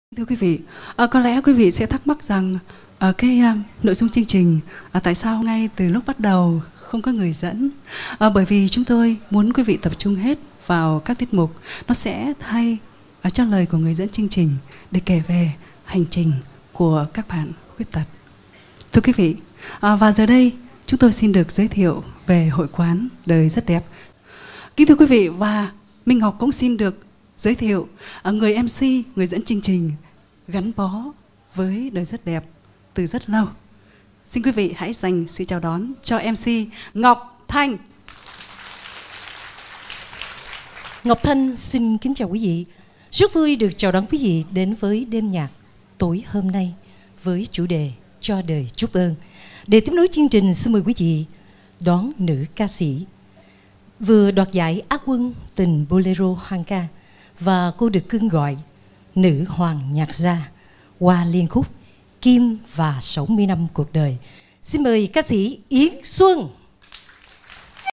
08 - MC Minh Ngọc giới thiệu MC Hội Quán Đời Rất Đẹp Ngọc Thanh | Đêm nhạc Cho Đời Chút Ơn
Tối 21/9/2018 vừa qua, Trung tâm Khuyết tật và Phát triển – Hội quán Đời Rất Đẹp (DRD) tổ chức đêm nhạc “Cho đời chút ơn” tại Nhà hát VOH Music One, số 37 Nguyễn Bỉnh Khiêm, phường Đakao, Q. 1, Tp. HCM. Chương trình nhằm gây quỹ học bổng “Người bạn đồng hành” hỗ trợ sinh viên khuyết tật có hoàn cảnh khó khăn đang theo học tại các trường đại học/cao đẳng. Học bổng Người bạn đồng hành đã và đang góp phần tạo cơ hội cho sinh viên khuyết tật học tập tốt hơn, trang bị kỹ năng sống, sự tự tin vào năng lực bản thân, từ đó góp phần thay đổi cách nhìn chung của cộng đồng đối với khả năng của người khuyết tật (NKT). Đồng thời, học bổng giúp các em có được cơ hội để hiểu thêm triết lý sống “Đáp đền tiếp nối” để đồng hành cùng DRD trong công tác trợ giúp NKT hòa nhập xã một cách đầy đủ và bình đẳng. Tính từ năm 2008 đến nay, đã có 388 sinh viên được hỗ trợ học bổng suốt 4 năm học đại học/cao đẳng. Nhiều em ra trường có đầy đủ kỹ năng, năng lực đáp ứng thị trường lao động, có vị trí nhất định trong xã hội.